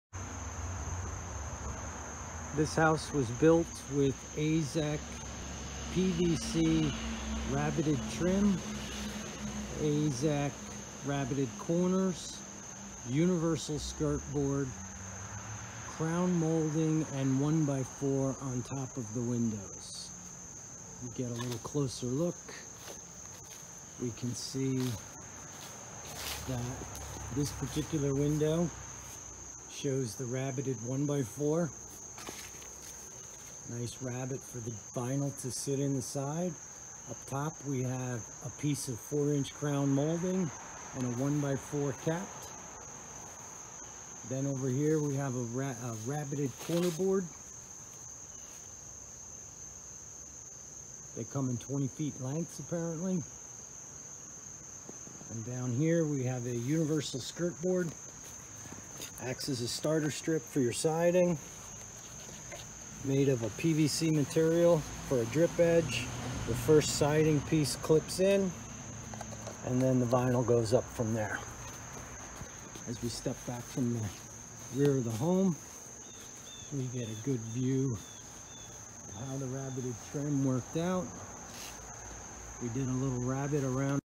AZEK PVC Universal Skirt Board and Rabbited Trim
This is a video showcasing a few of the many AZEK trim materials. OX Construction in Northern NJ has been installing AZEK Trim for years. Many of the homes built by OX look different because they use all the many products made by AZEK Building Materials. showcased here is 5/4" rabbited 4" trim boards, Universal Skirt Board and Rabbited corners to go with Vinyl Siding. This will dress up the look of any home and make it POP better much than vinyl J channels and corners. WOW! What a difference a little AZEK makes.